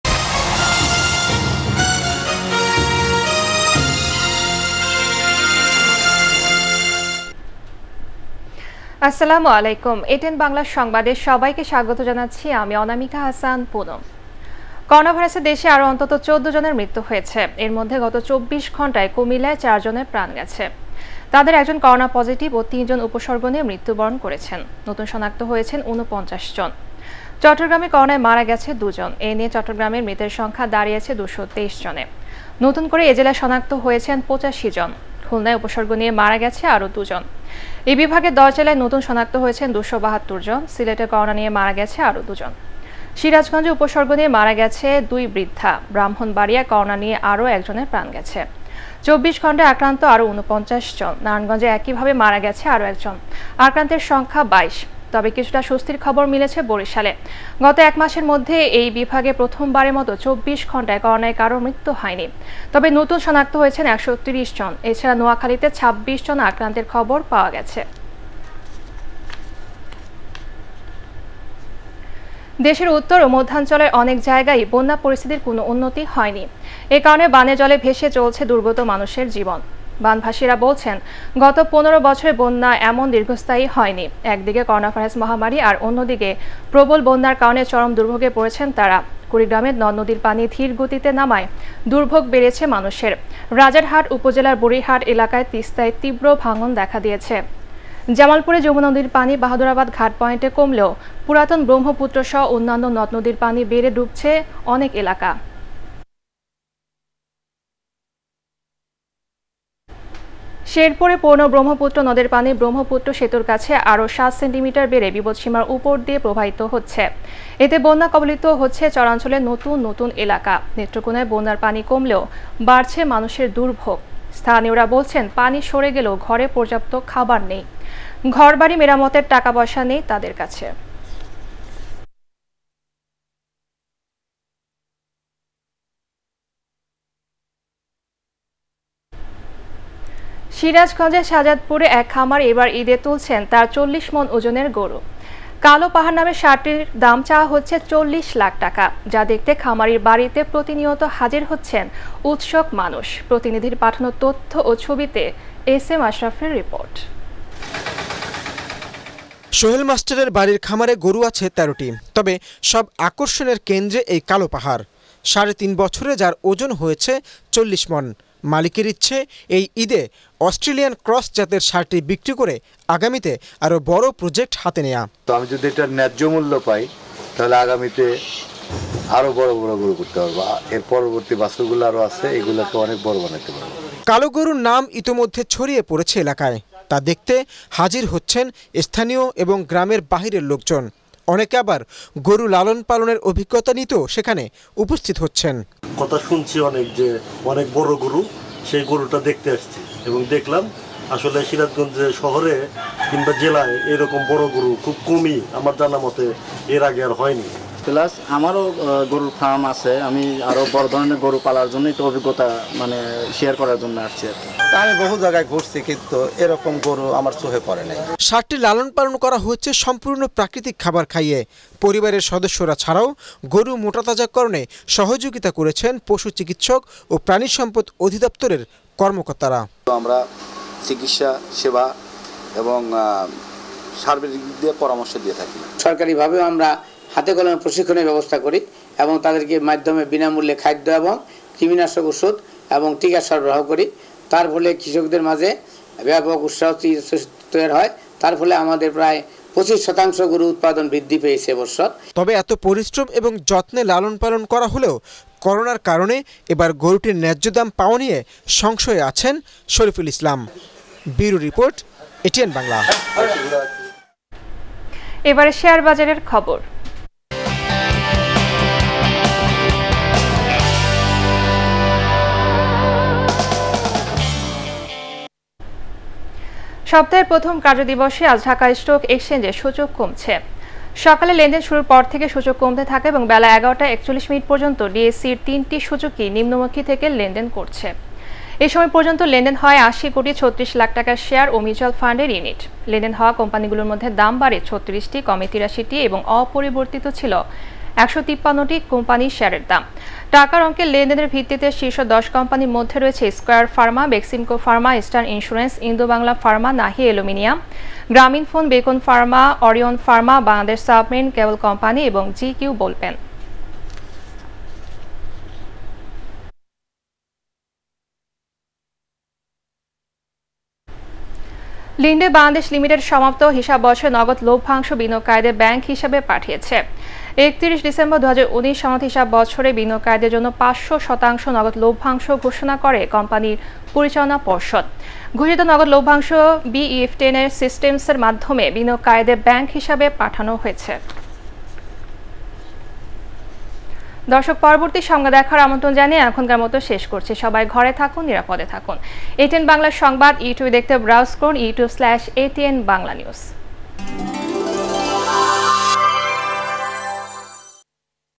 এটিএন বাংলা দুপুর ১২ টার সংবাদ। 19.07.2020 | ATN Bangla News at 12 pm
News Presented by- Onamika Hasan Punom